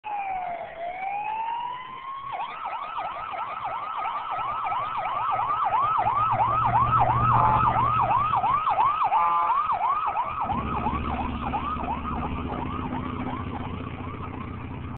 Theresa fire department 3481 with 6X6.3g2
Knowles Fire Department had a field fire in the town of Farmersville. Kekoskee sent their tanker and grass rig and Theresa was called for their grass rig, 3481 and 6x6. Theresa WI.